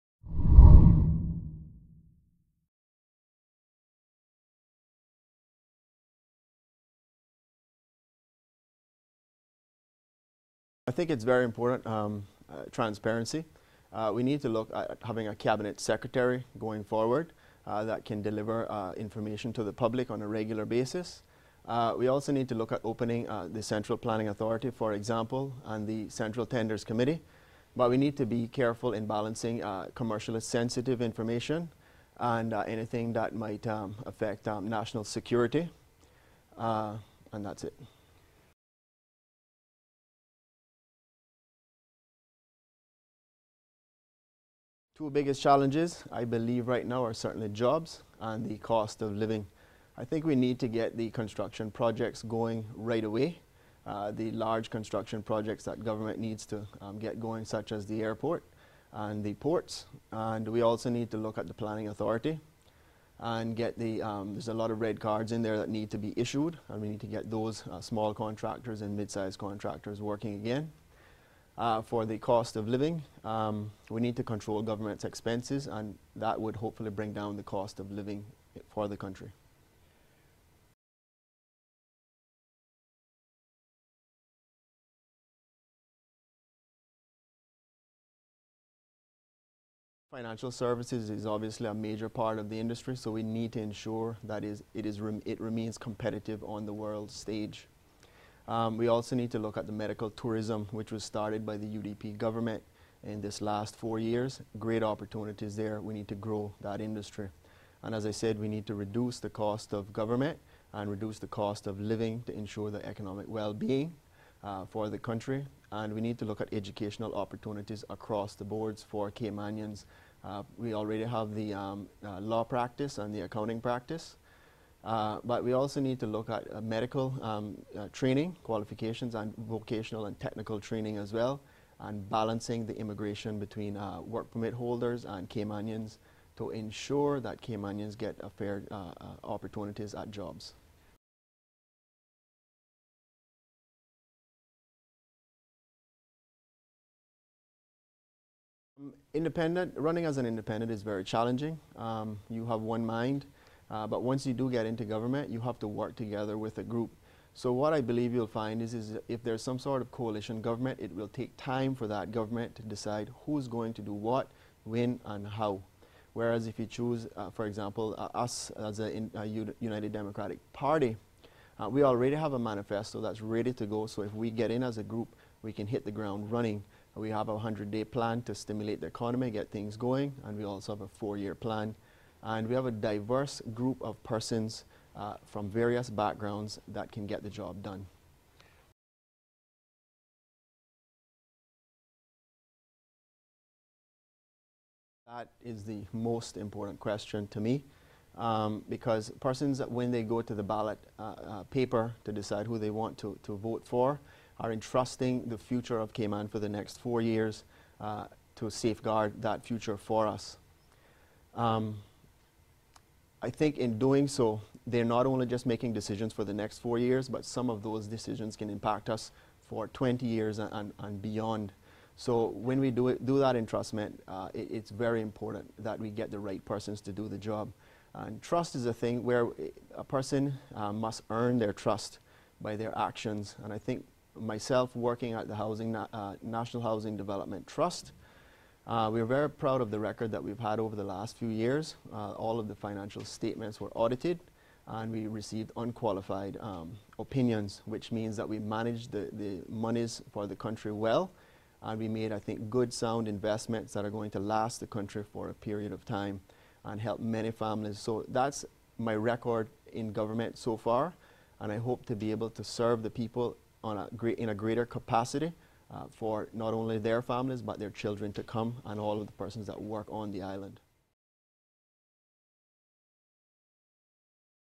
Elections Rayal Bodden
United Democratic Party candidate for George Town, Rayal Bodden answers questions on Compass Election Coverage 2013.